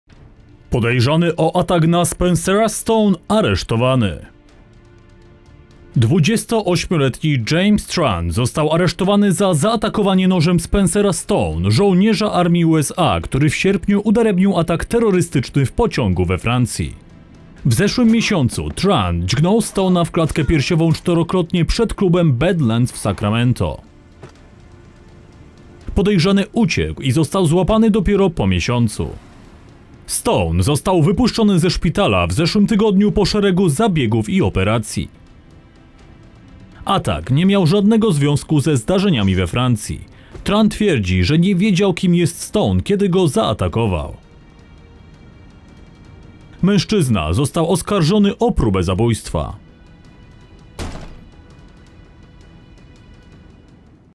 Podejrzany o atak na Spencera Stone aresztowany
28-letni James Tran został aresztowany za zaatakowanie nożem Spencera Stona, żołnierza armii USA, który w sierpniu udaremnił atak terrorystyczny w pociągu we Francji.
W zeszłym miesiącu Tran dźgnął Stone'a w klatkę piersiową 4-krotnie przed klubem Badlands w Sacramento.
Podejrzany uciekł i został złapany dopiero po miesiącu.
Stone po operacji został wypuszczony ze szpitala w zeszłym tygodniu po szeregu zabiegów i operacji.
Atak nie miał żadnego związku ze zdarzeniami we Francji. Tran twierdzi, że nie wiedział kim jest Stone kiedy go zaatakował.
Mężczyzna został oskarżony o próbę zabójstwa.